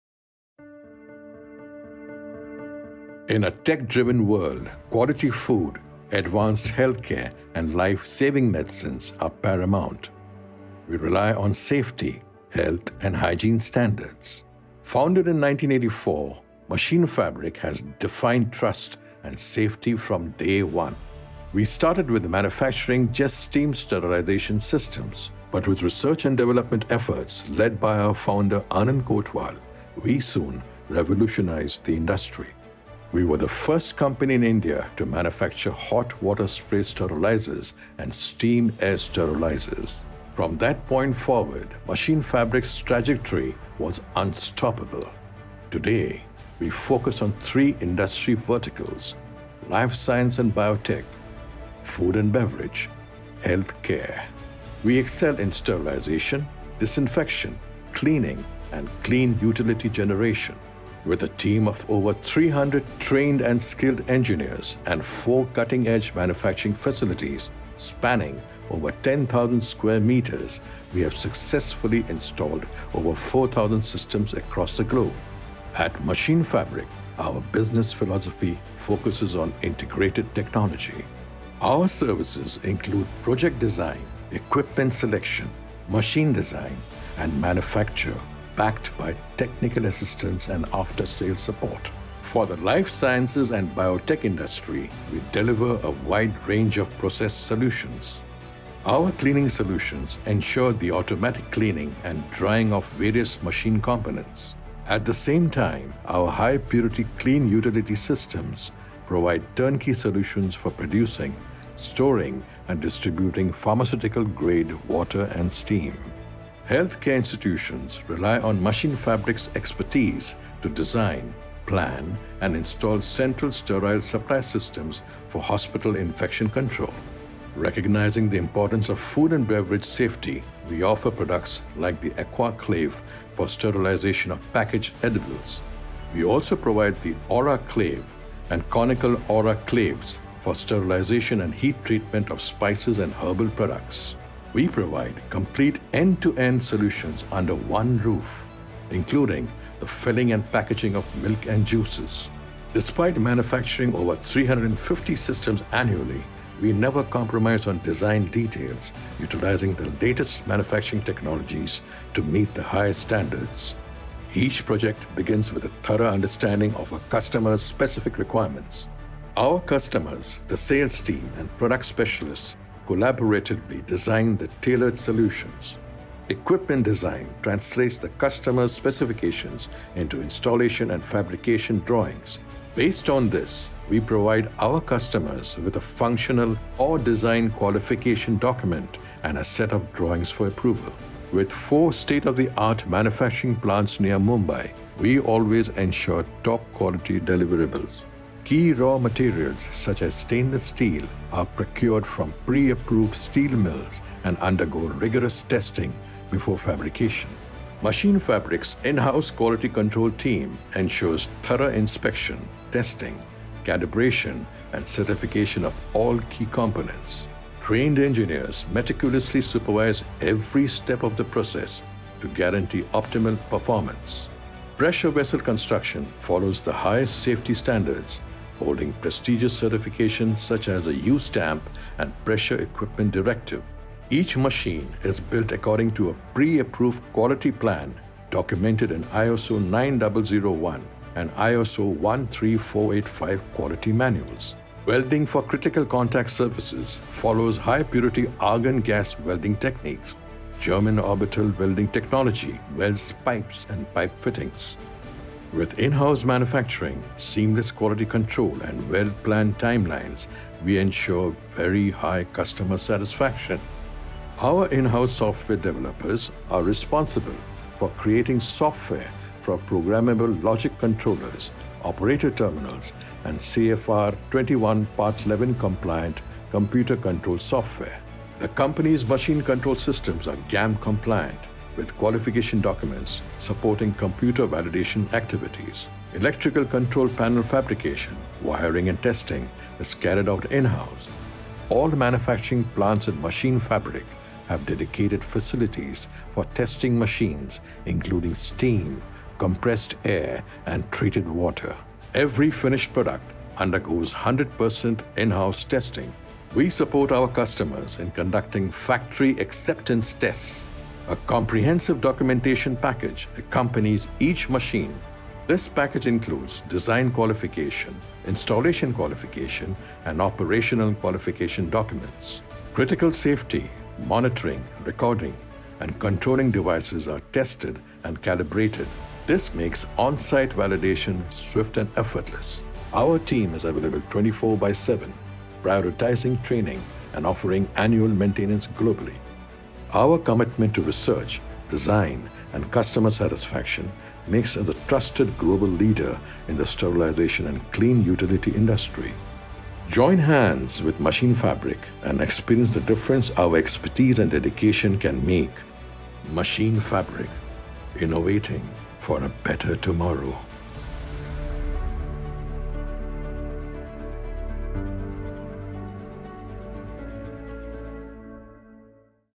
Machinfabrik Corporate film
Established in 1984, MACHINFABRIK INDUSTRIES is renowned manufacturer of sterilization & decontamination solutions for the Pharmaceuticals, Hospitals and Food & Beverage sector.  As an expert in sterilization and infection control solutions, Machinfabrik has been supplying a range of top-quality sterilizers, washers and disinfectors to the various customers of Pharmaceuticals, Hospitals and Food & Beverage sector.
Founded by Mr. Anand Kotwal in small set-up years ago, the company has grown in to 4 state-of-art manufacturing units to cater the growing demands from the customers.
A 40-year-old company that has its performance speak for itself with 4500+ successful installations in 45+ countries, Machinfabrik presents a wide range of sterilization equipment & has several innovations to its credit.
Machinfabrik’s ideas are based on Coordinated Technology to offer complete solutions at different levels; project design, equipment selection, machine design & manufacture, technical assistance and after sales services. The basis for coordinated technology is finding the needs of the customer and analysing requirements thus creating a back-ground of ideas which eventually shape into a prototype for thorough trials and leading to the final product. It is with this philosophy that Machinfabrik has made its presence known with optimum solutions for the diversified needs of its customers.